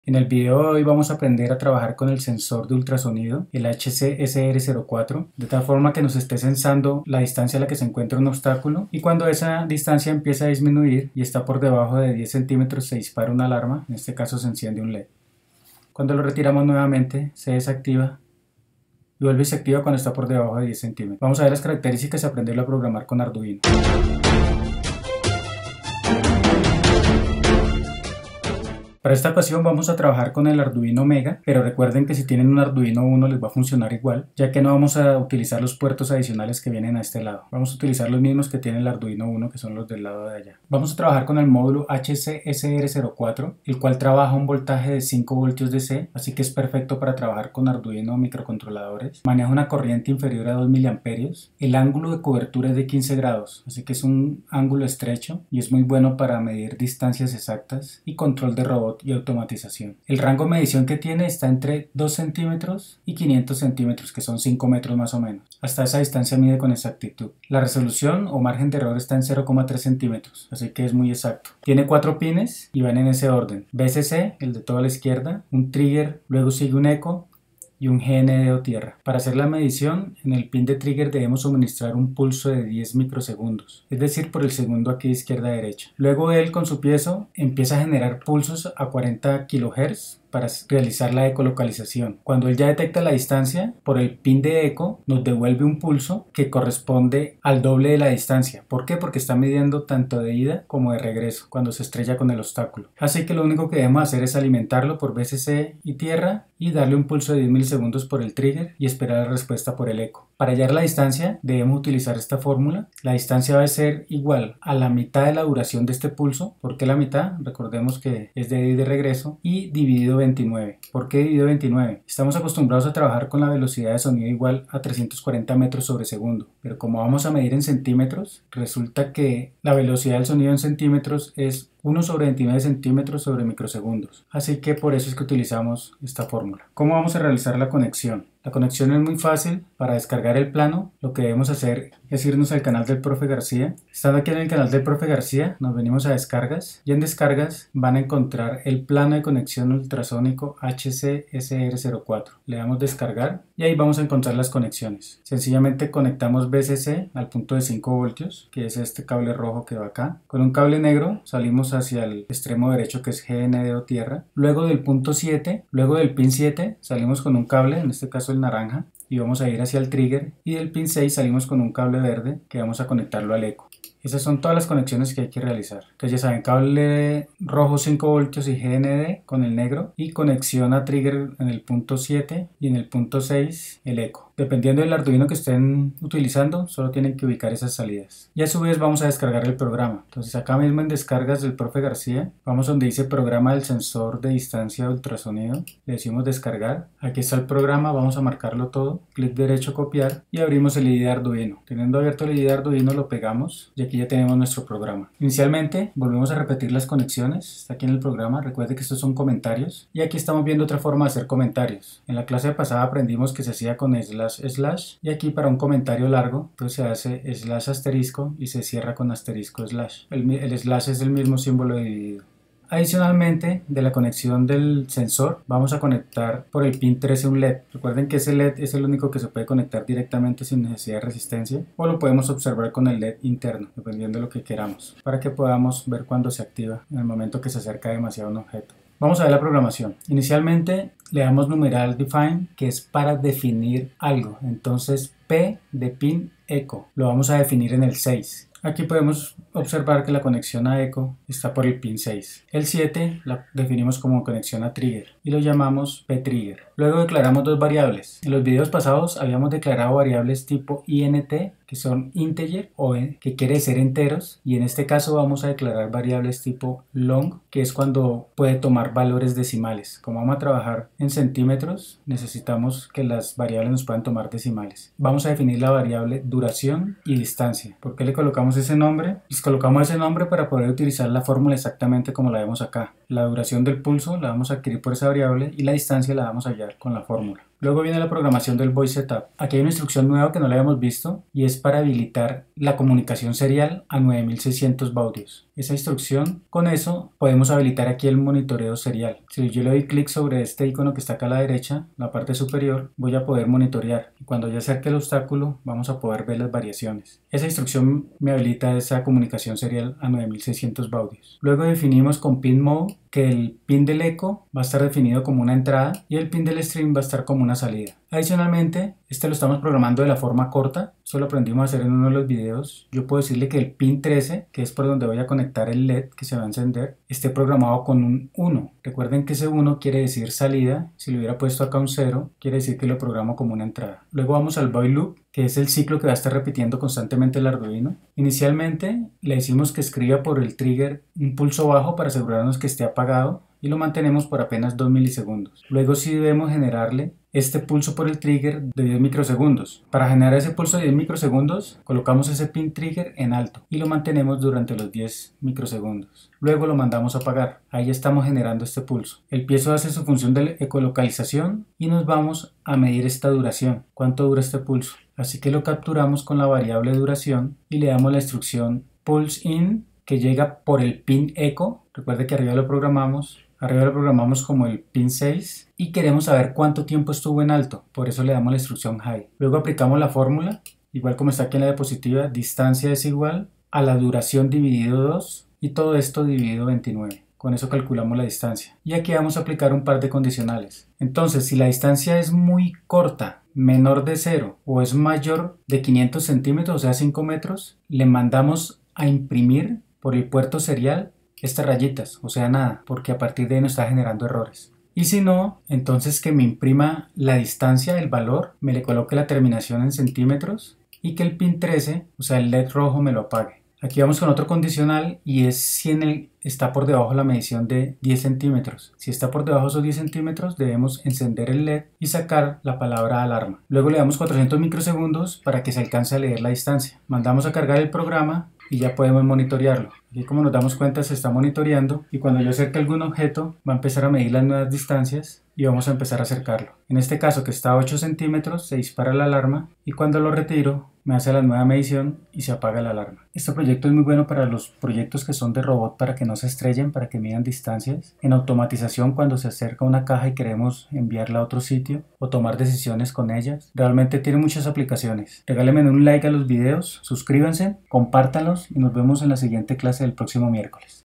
✅ Sensor de Distancia (proximidad) hasta 5 metros / HC-SR04 y ARDUINO
HC-SR04 sensor ultrasonido, como se programa con Arduino

(1) Arduino UNO
(1) Sensor Ultrasonido HC-SR04
(1) Mini Protoboard
( ) Cables de conexión